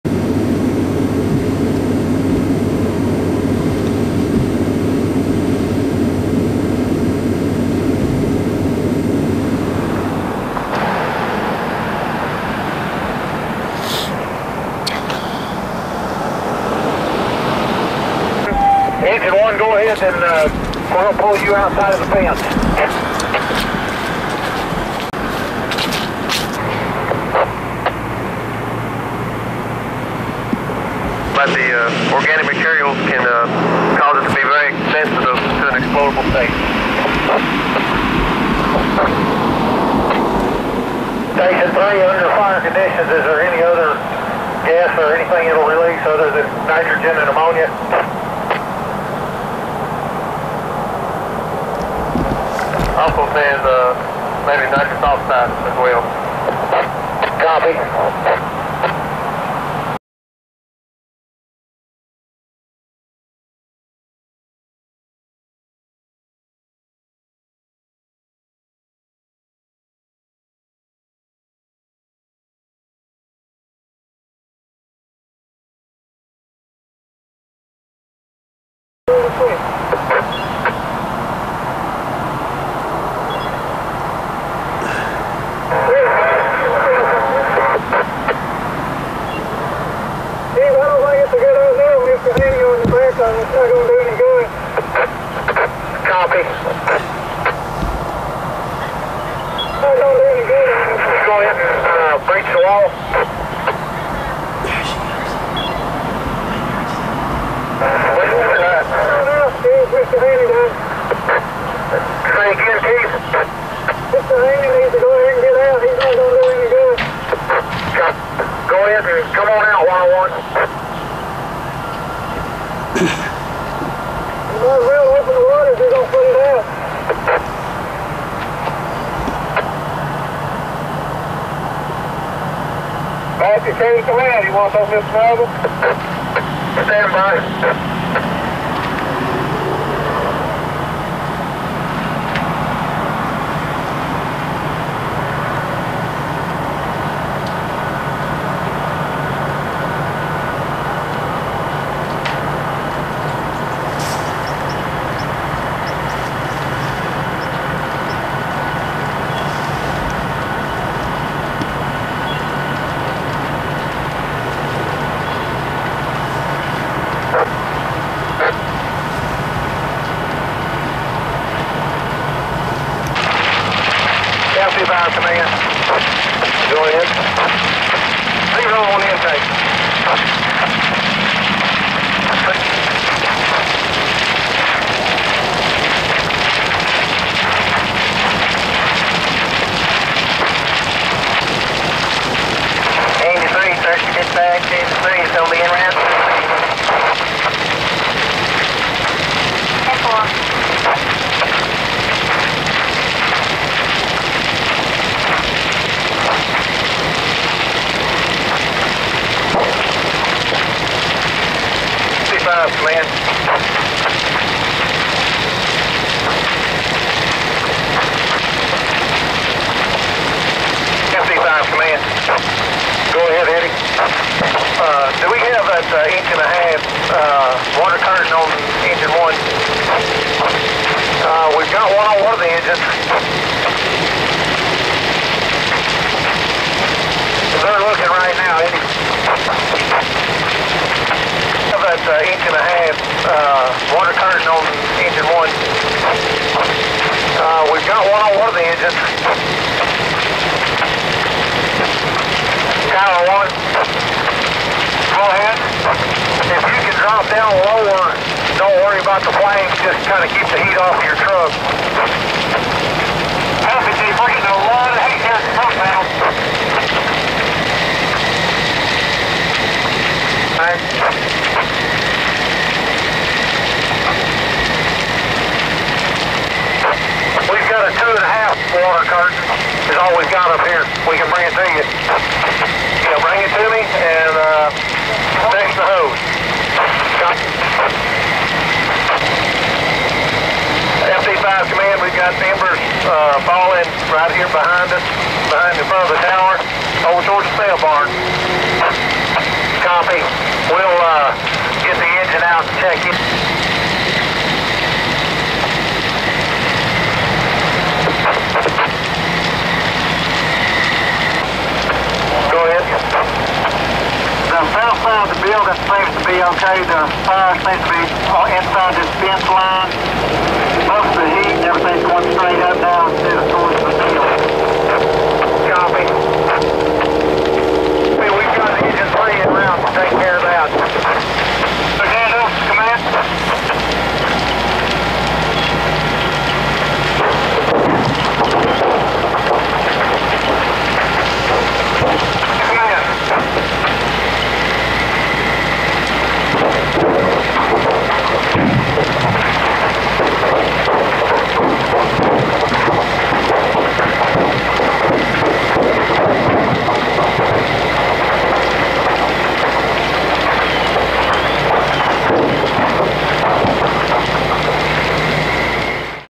Ammonia nitrate fire burns a metal building to collapse in HD
Reporter Darron 4-A-Day captures an amazing fire progression in true HD. Location is Searcy Arkansas. Watch as this nitrous oxide fueled fire melts metal beams to liquid and nearby telephone poles ignite. All right next to a very populated apartment complex. The city was covered with putrid smelling fumes and ashes.
Visit searcypress.com for the longer video.